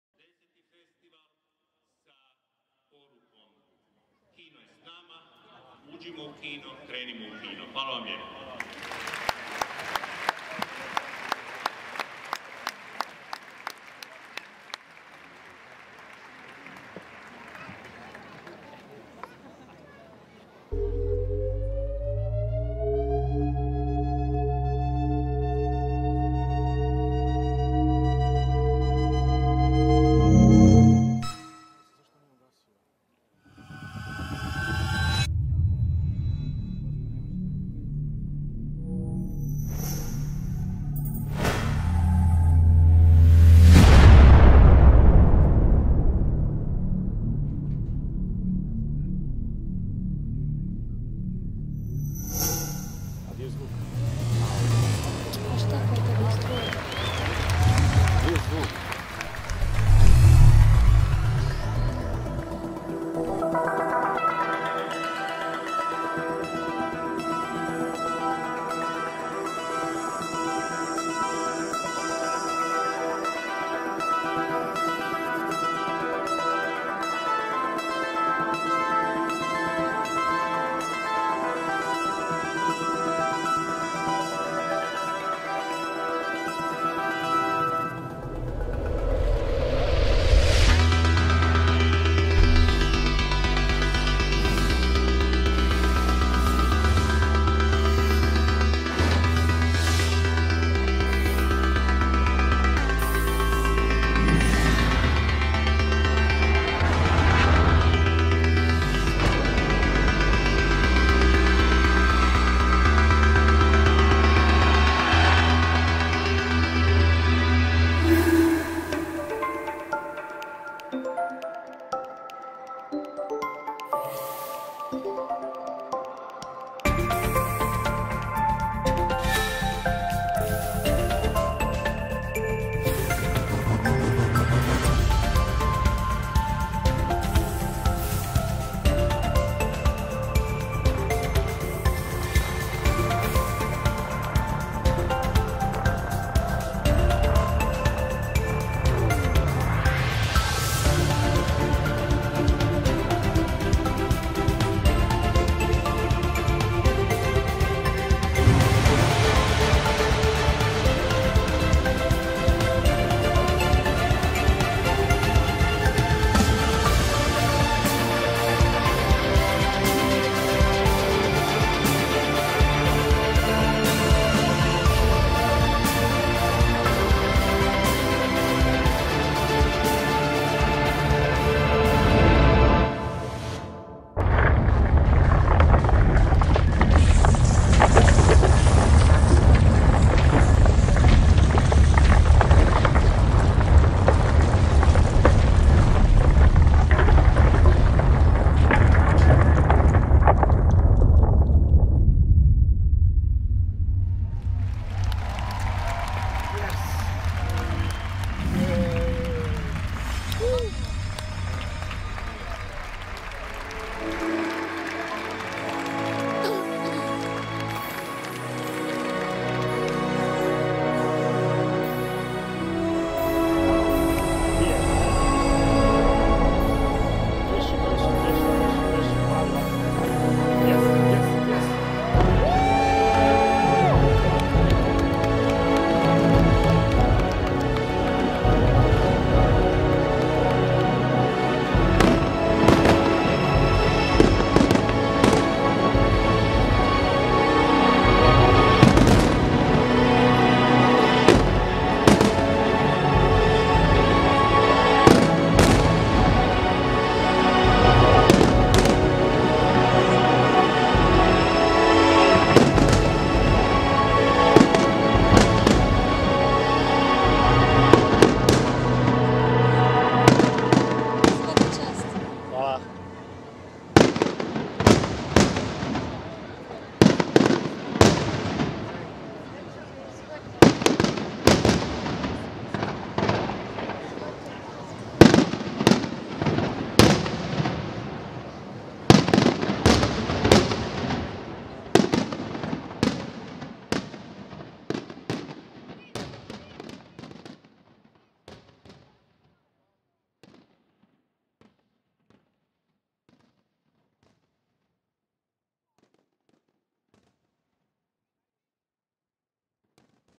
60th Pula Film Festival Opening
3D Video Mapping in the Pula Arena for the opening of the 60th Film festival in 2013
Music & Sound design: Kyril Rubinstein